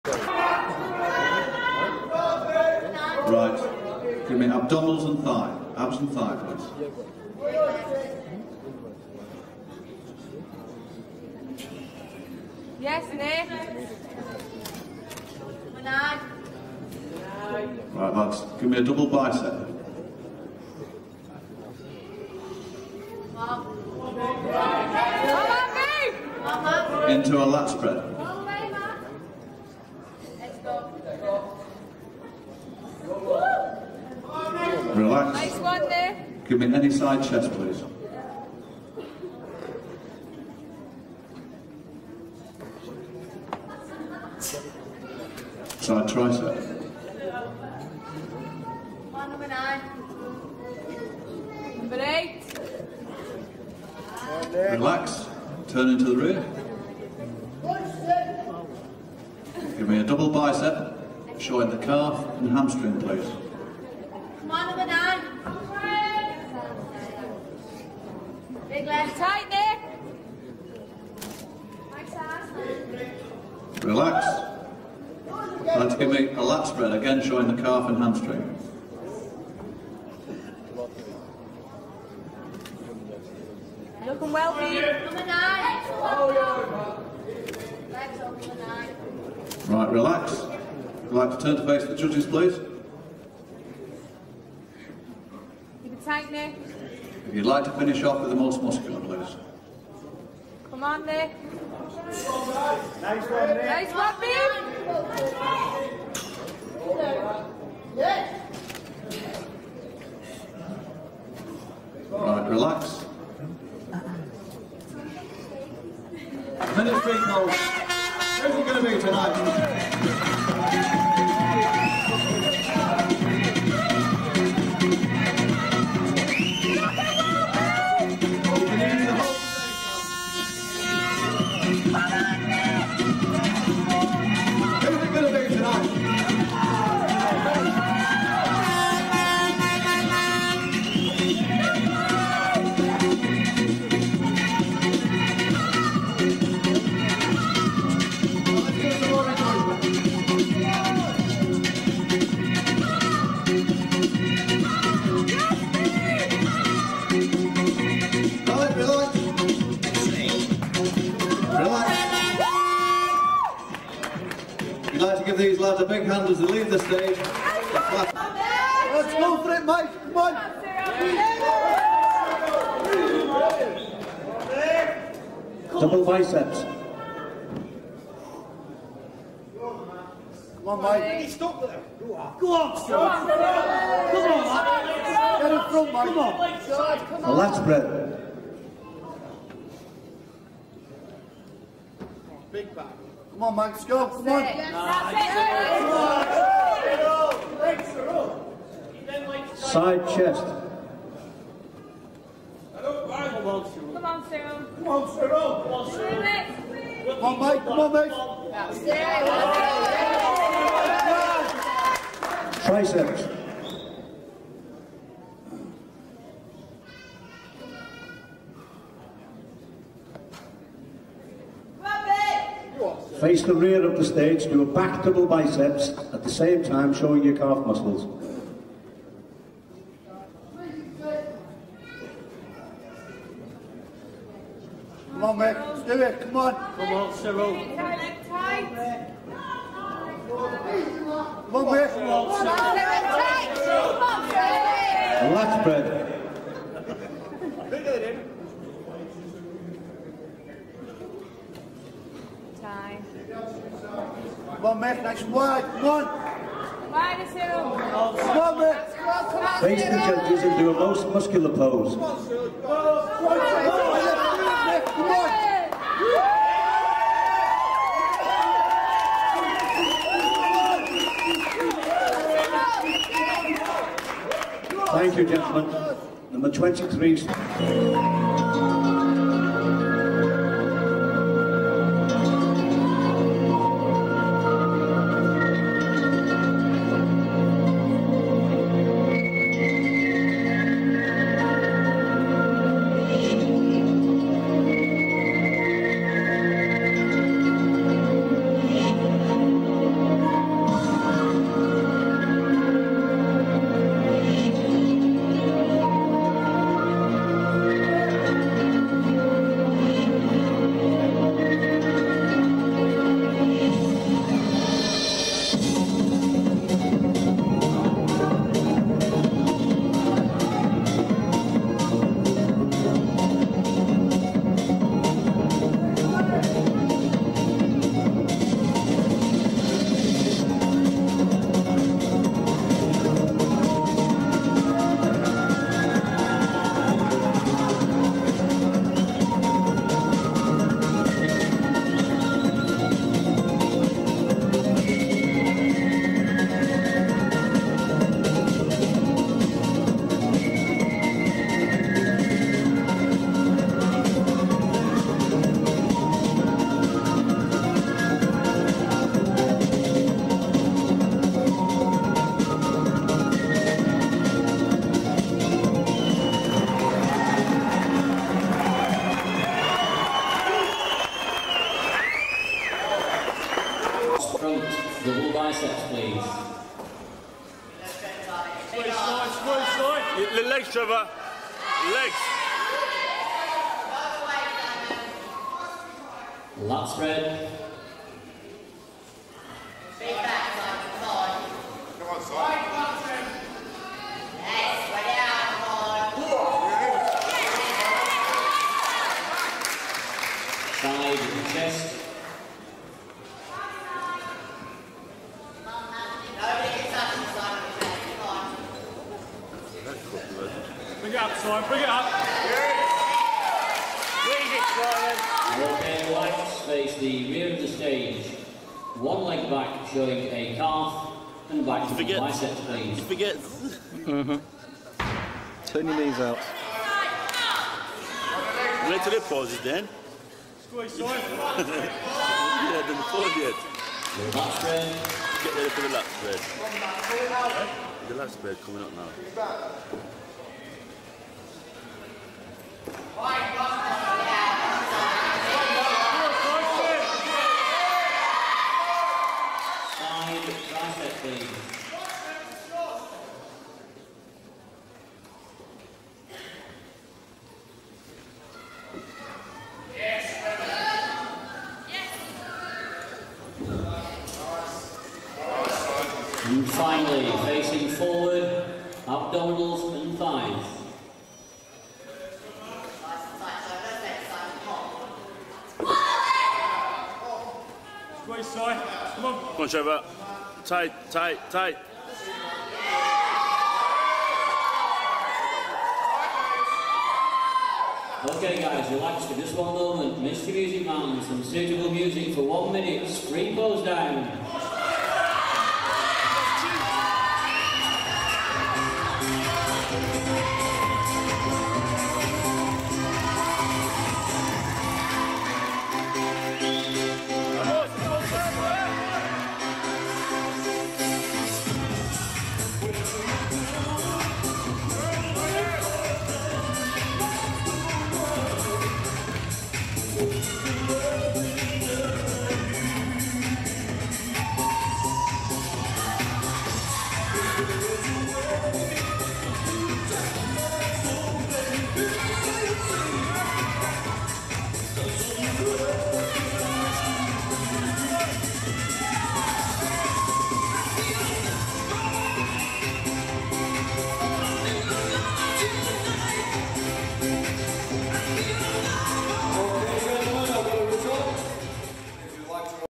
Matthew Thompson 1995,1996,1997
Matthew Thompson in the NABBA NW 1995 & 1996. then in the EFBB 1997